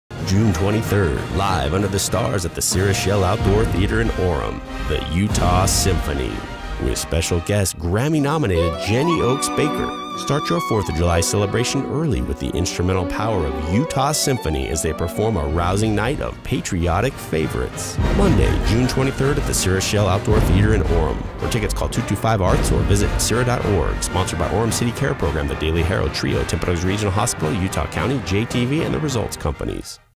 06/23/14 Utah Symphony Jenny Oaks Baker
Utah Symphony With Grammy - Nominated Jenny Oaks Baker
Monday June 23, 2014at the SCERA Shell Outdoor Theatre